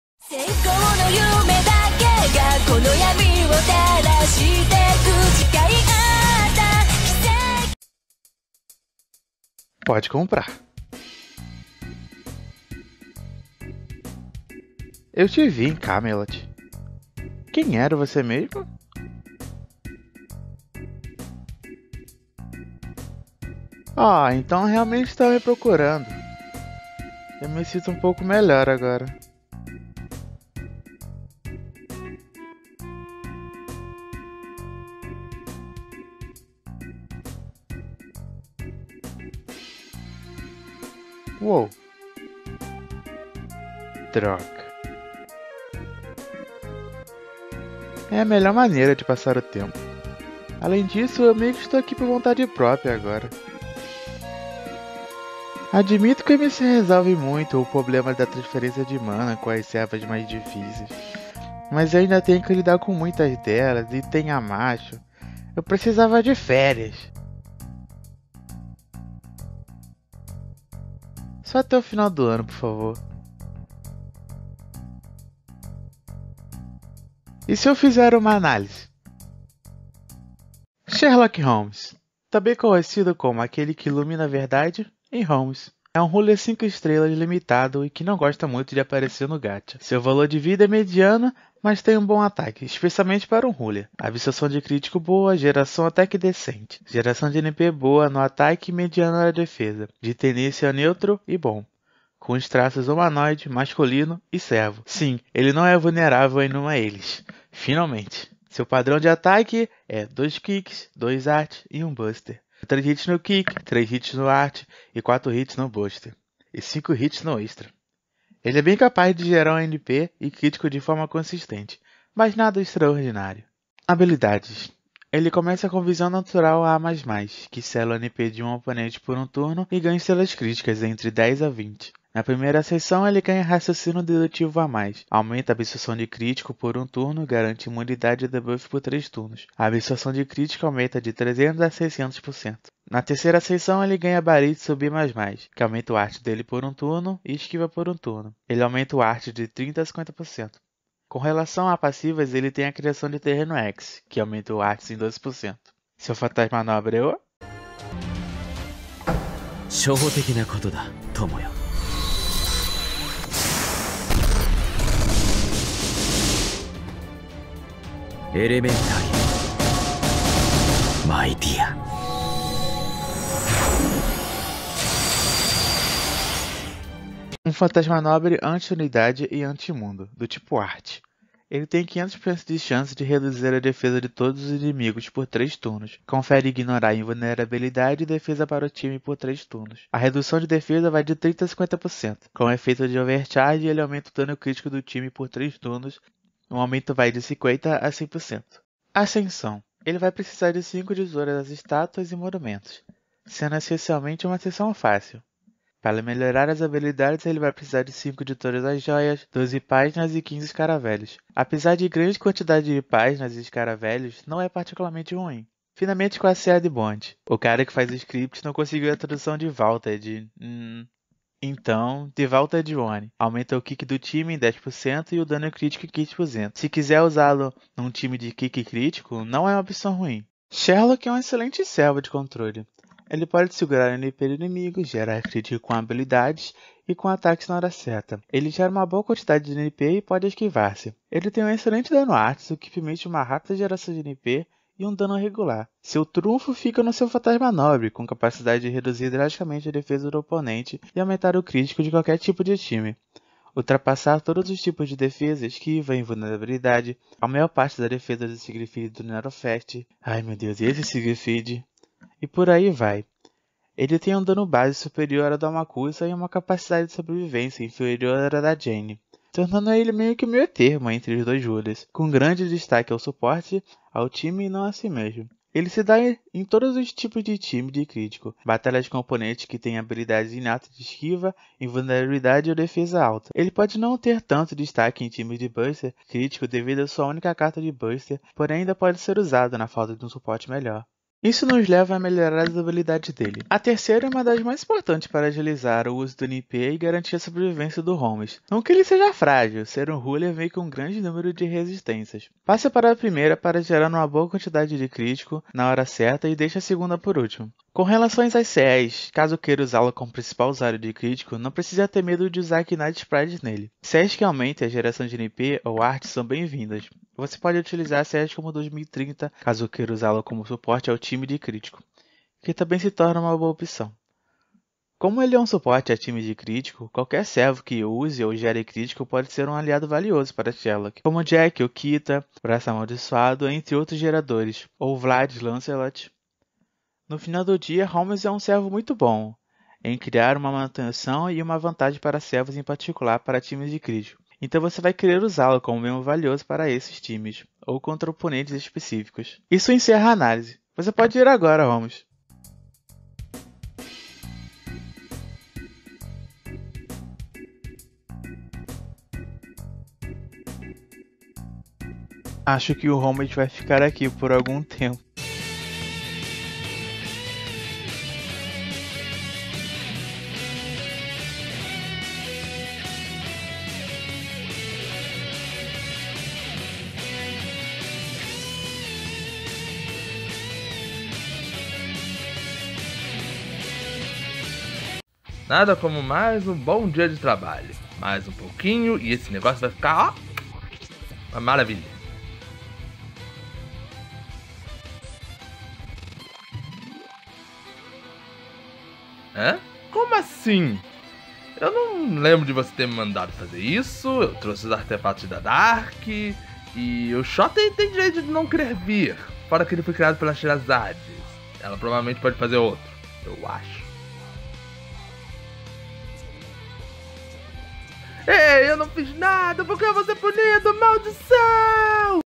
Análise de Servo - Holmes
O quão bom é o maior detetive do mundo? N-não... eu não estou falando do Batman... também não estou falando do Lucifer... não também não é Scooby Doo e sua turma... é Sherlock Holmes!